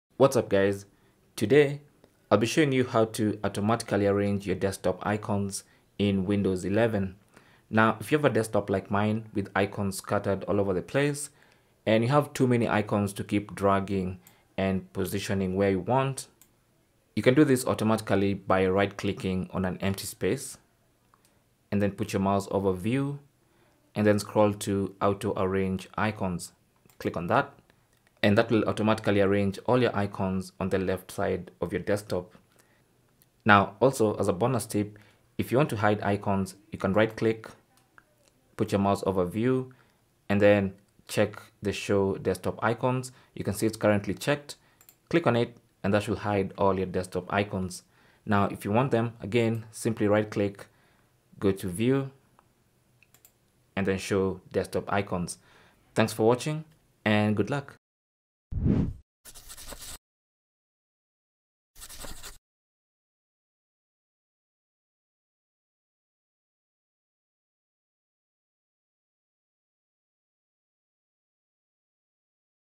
Windows 11: How To Automatically Arrange Desktop Icons | Organize Desktop Icons
Learn how to auto arrange desktop icons on Windows 11. If you're tired of arranging desktop icons every time, you can now simply automatically organize the icons in Windows 11. With this, all icons will be placed in an orderly manner.

This can be done on a computer and any laptop that runs Windows 11.

Timestamps
0:00 - Intro
0:07 - How to automatically organize desktop icons in Windows 11
0:55 - Outro

Windows Specifications
Edition: Windows 11 Pro
Version: 21H2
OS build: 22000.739

Cell Phone Tripod Adapter